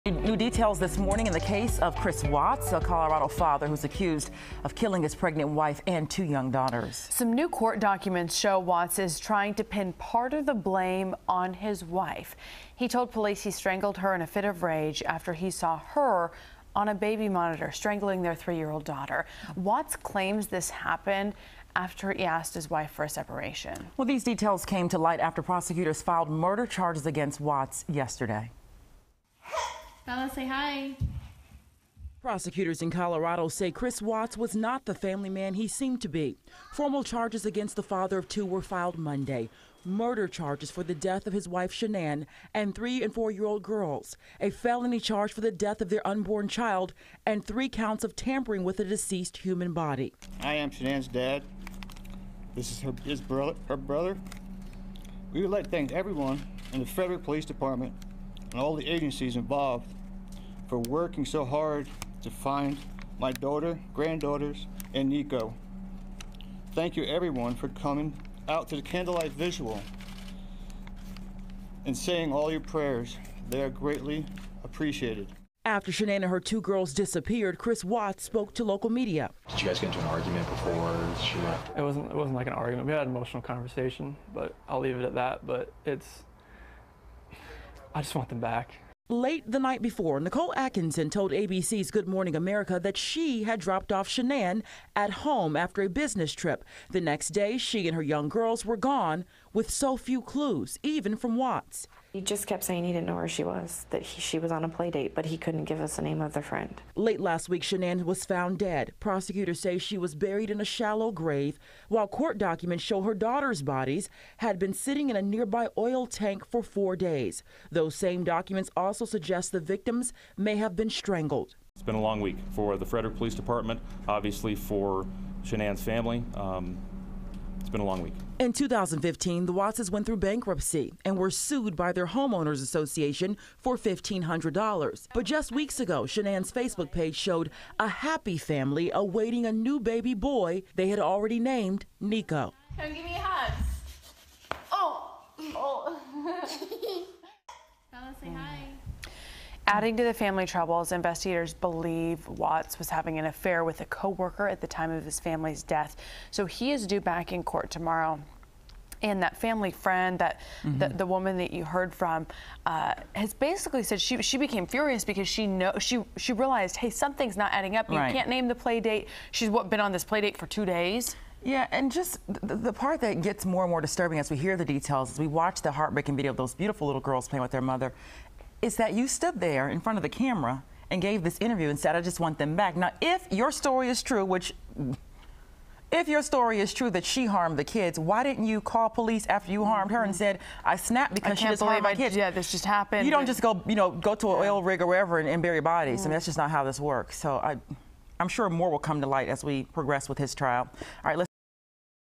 Watts Affidavit Reveals New Info
New details in the case of Chris Watts, the Colorado man accused of killing his pregnant wife, Shanann, and two young daughters.